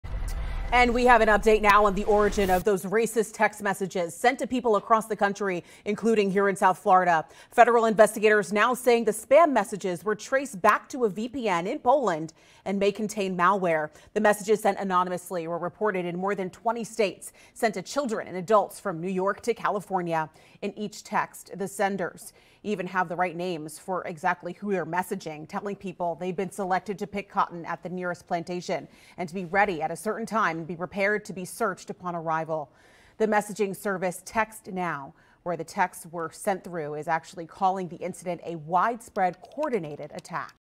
Racist text messages sent across the US traced back to Poland
Federal investigators are now saying that racist text messages sent to people across the U.S., including in South Florida, were traced back to a VPN in Poland and may contain malware.